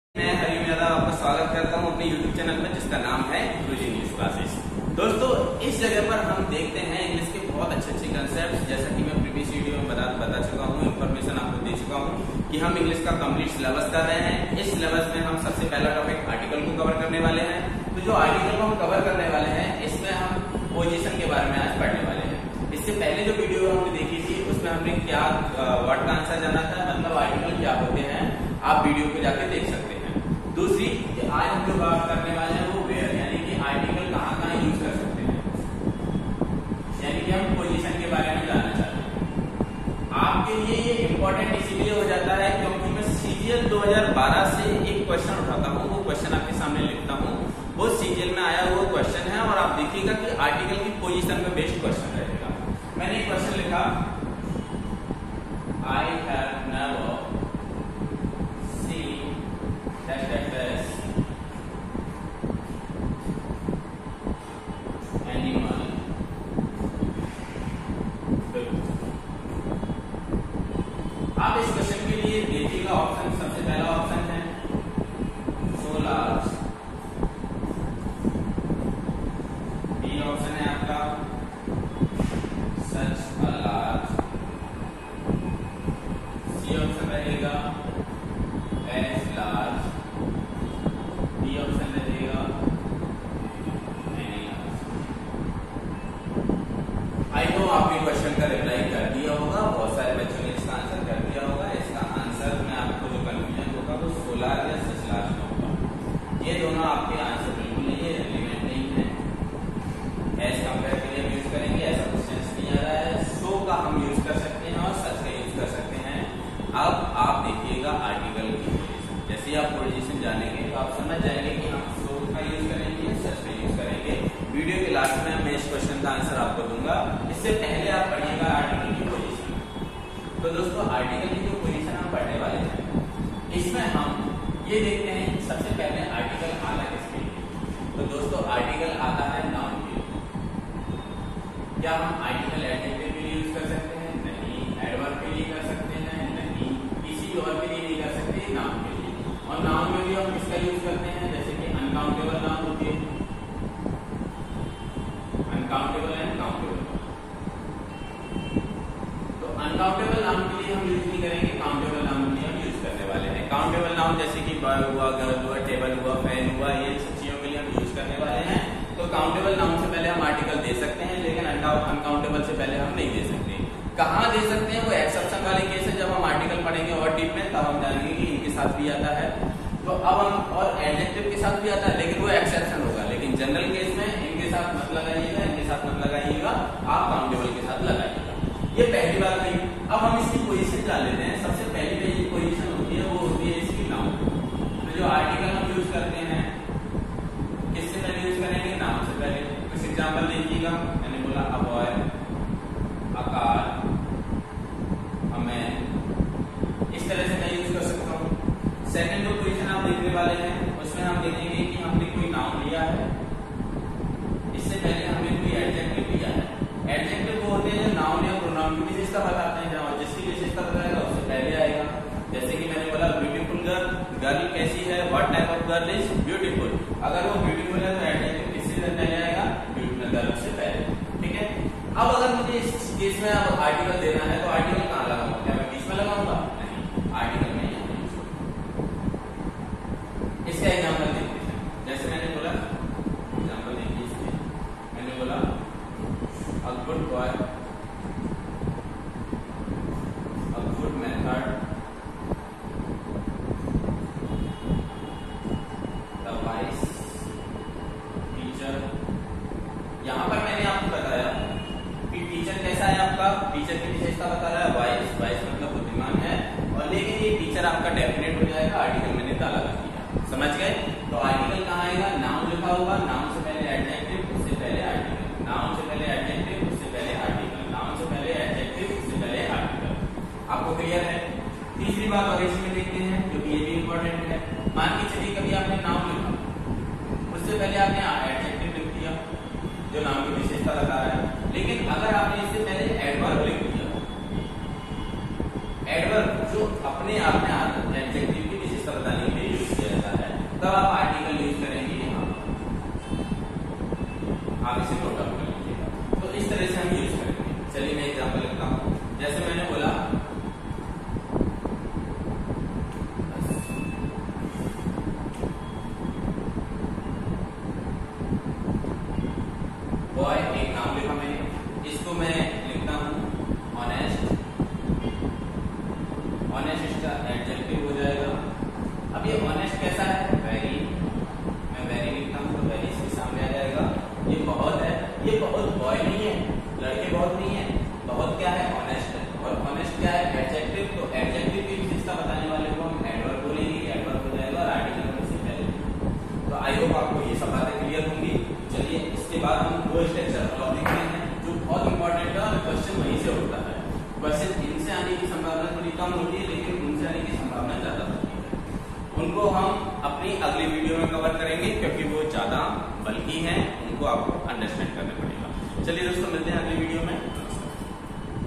Article basic part 2 , SSC, NAVY , AIRFORCE By Guruji classes , Harry sir
Dear students boost up your knowledge about English grammar , learn article and improve your basics if any doubt comments me